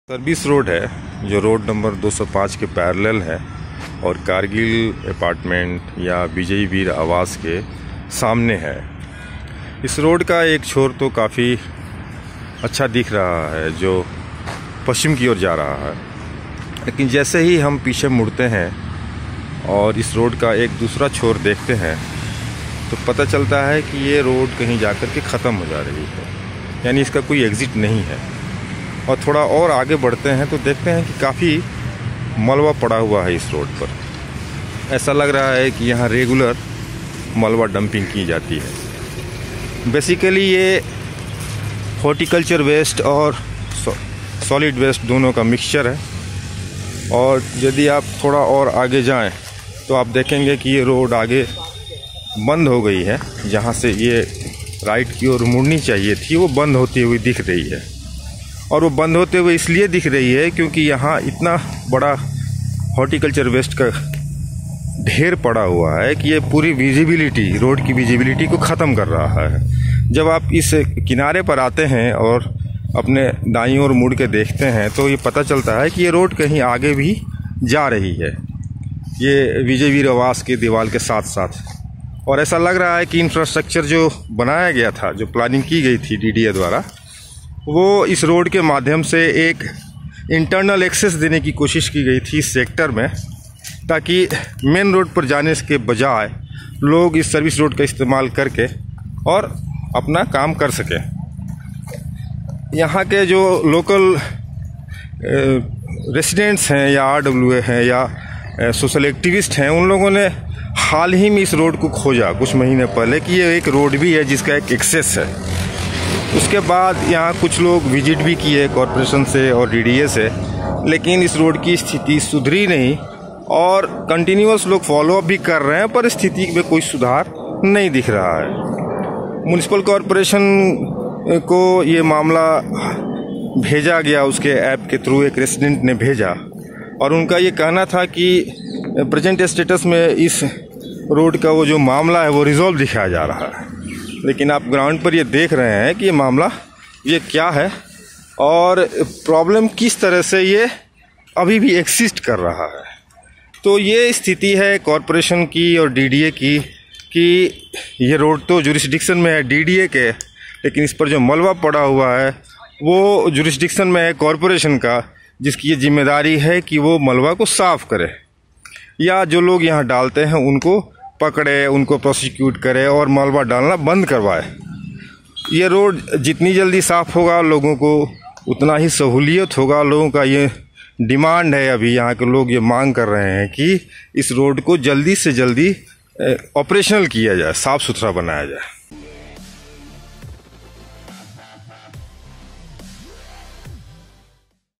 A service road in Dwarka that ends 'abruptly'
The video shows the deplorable condition of a service road, located in Sector-18 of Dwarka